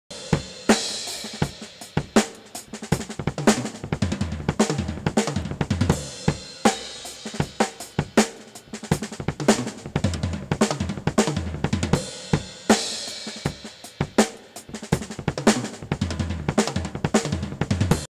드럼 8연음 필인 연습
연습 모니터링 하려고 찍은 영상 중 한 부분입니다